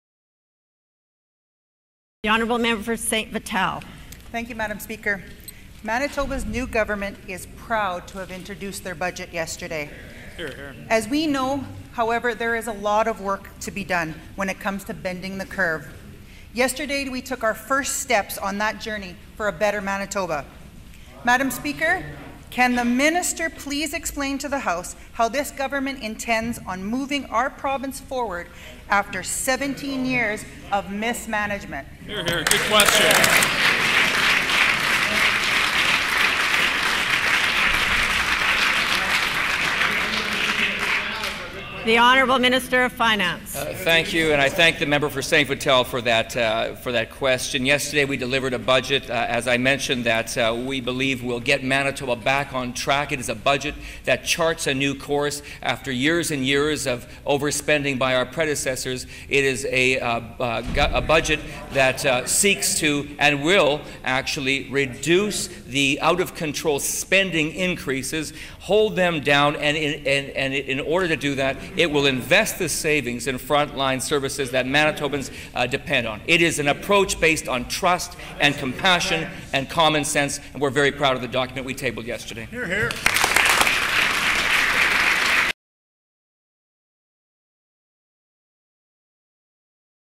Colleen Mayer asks Finance Minister Cameron Friesen about Budget 2016
St. Vital MLA asks Finance Minister Cameron Friesen about the government's plans in Budget 2016.